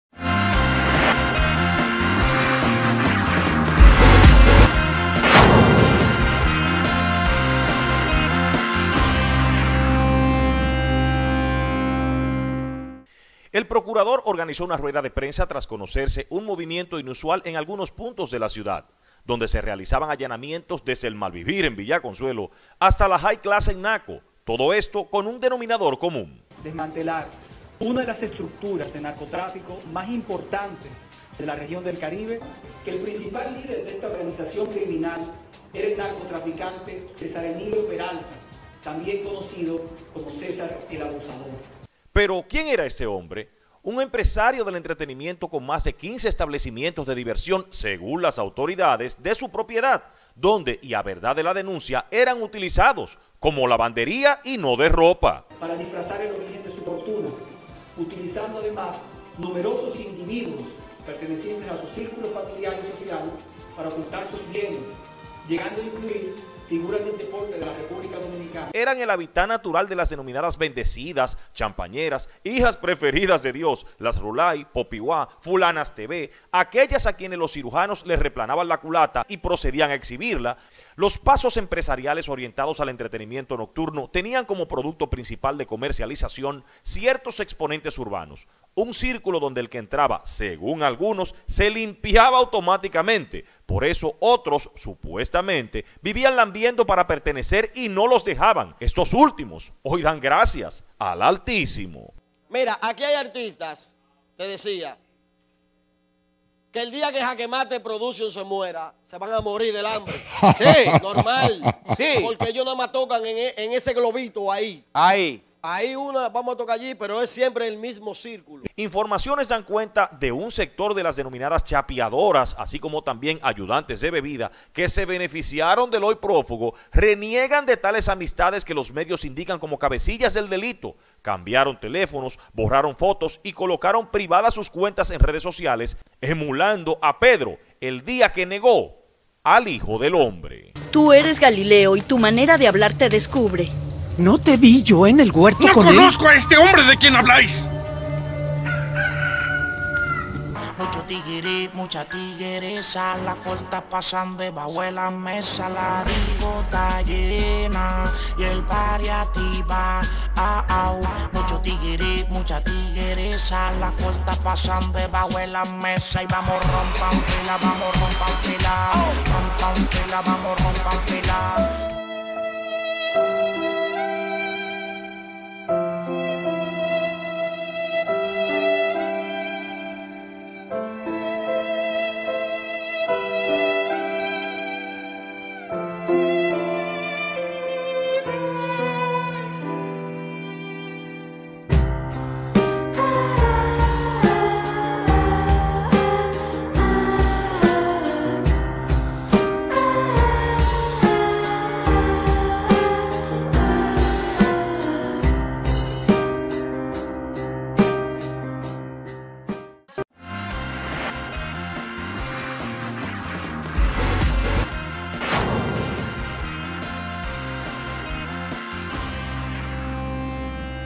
Ahora, Nadie conoce a Cesarelabusador?
Luego de los allanamientos y declaraciones de las autoridades sobre apresamientos, mucha gente que presuntamente se beneficio de las circunstancias actuando como Pedro el que negó a Jesús ahora dice no conocer a los protagonistas de esta historia.

Según fuentes desde artistas hasta chapiadoras, andan muy preocupados porque ya no tendrán donde picar, en iguales términos de tristezas estan varios DJS "que ellos si"  sin contar con los camareros de hooka, seguridad, bartenders, damas de para bulto en los VIP parqueadores, propietarios de picalongas, chimichurreros y maipilos, algunos que hasta prestamos bancarios tenían, ahora no saben como salir de estos compromisos.

Si bien es cierto que este tipo de situaciones tienen un componente legal, también lo es que llevan otro de carácter social y humano.